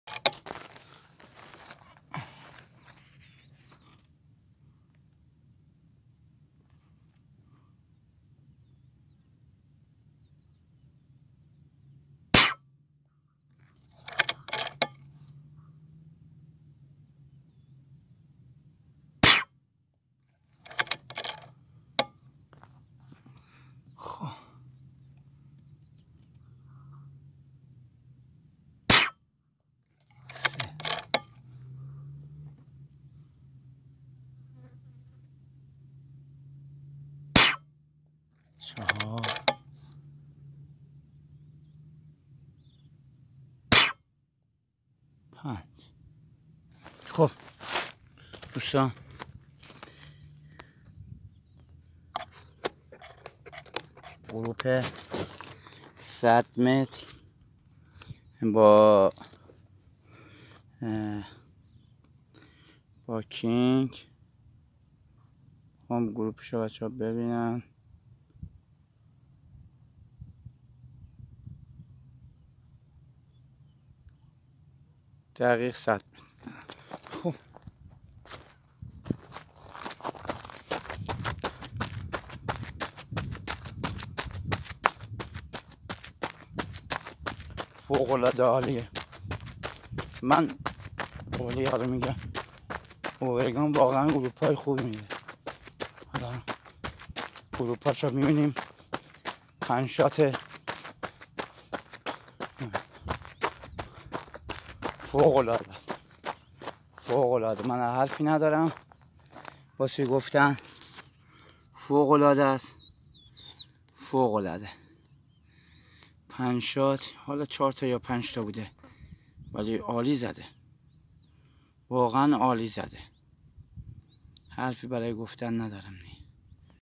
Airgun technology uragan king
Uragan king
100m
Jsb pellet 18 grain
5shot group
Location iran tehran
Shooter:sayad nasr abady
Element taitan 5/25/56 ffp
Click 1/4 moa
Without silencer
60cm, barrel
Season summer
Rangefinder MTC